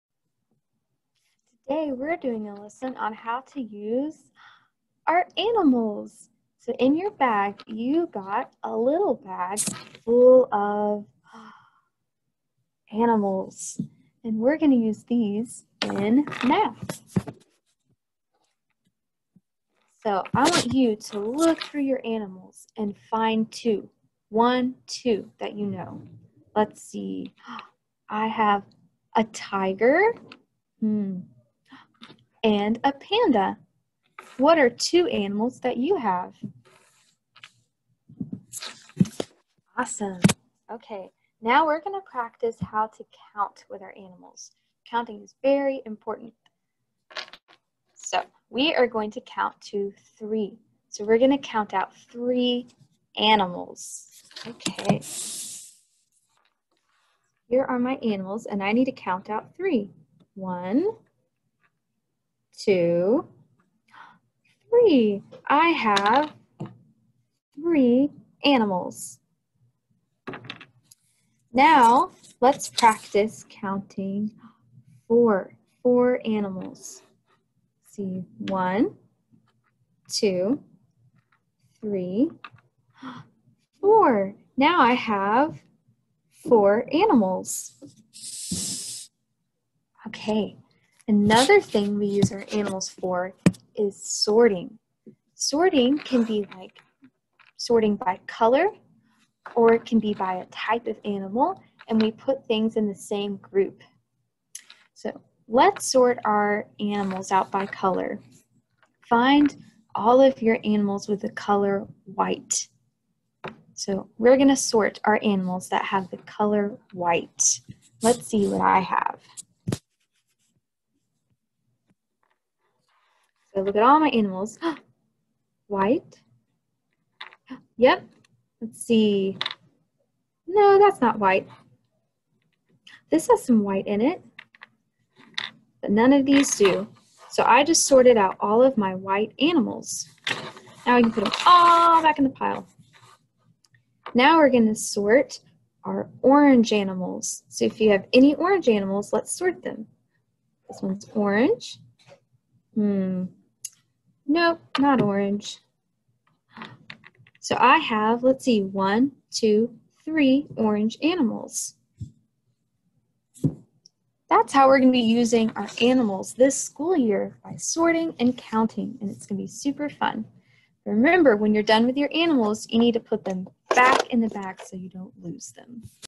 September 8-11: Using Manipulatives
In this lesson, I model how to use the animal manipulatives. We practice counting and sorting by color!